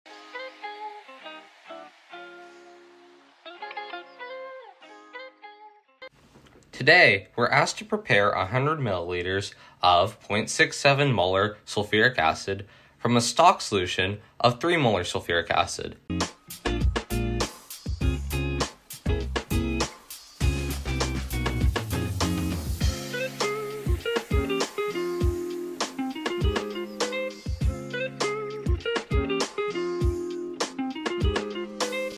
You are instructed to produce solutions of 0.67M H2SO4 and 0.87M NaOH for your lab experiment. You …
You are instructed to produce solutions of 0.67M H2SO4 and 0.87M NaOH for your lab experiment. You will be provided with a 3.0 M stock solution of H2SO4 and solid NaOH. You will need to make 100 mL of each solution. How many mL of H2SO4 stock solution will you need to make the 100 mL of H2SO4 Solution at 0.67M?